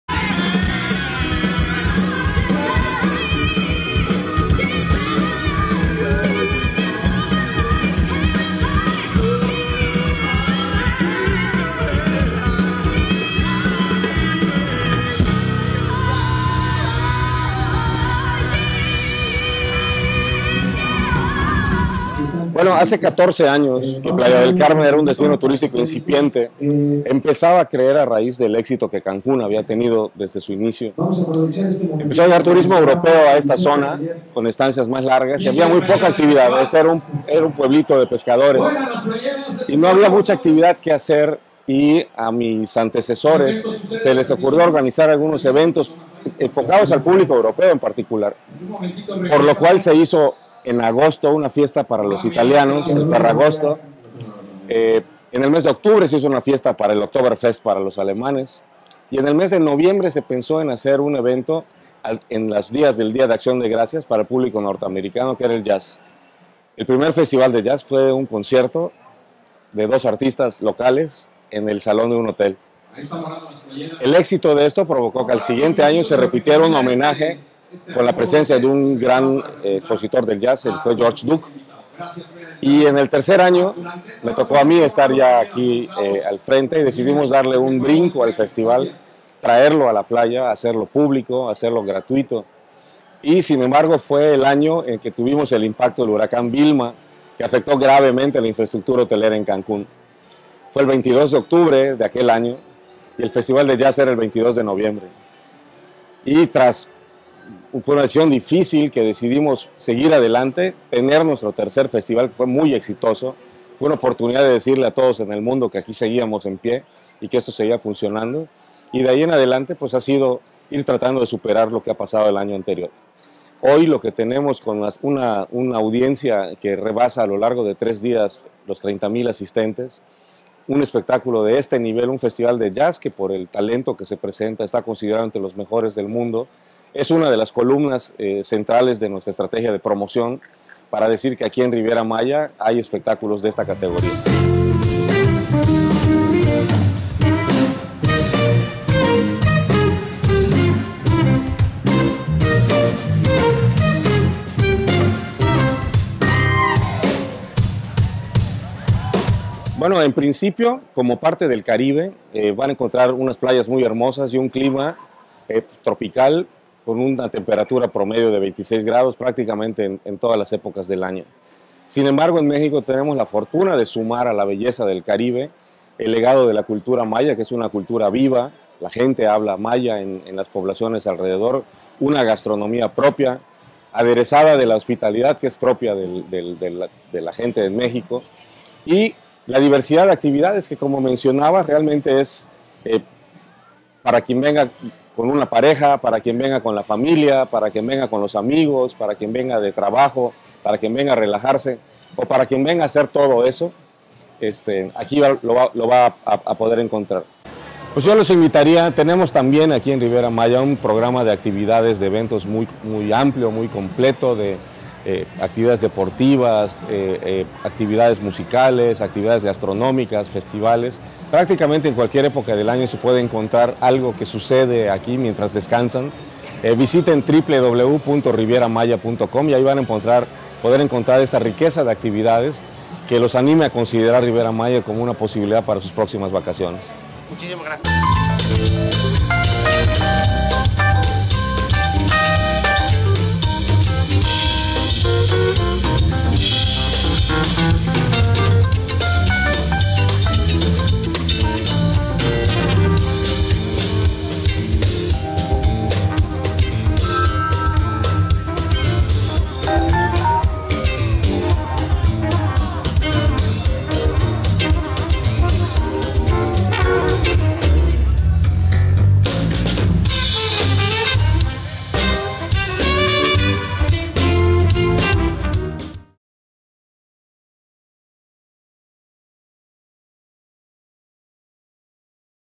Dario Flota: "El festival de Jazz de Riviera Maya está considerado entre los mejores del mundo"
El director del fideicomiso de Riviera Maya, Dario Flota, es una de la personas que lideran la organización y que hacen posible que este festival de Jazz sea considerado uno de los mejores del mundo. Hemos podido entrevistarle durante el festival.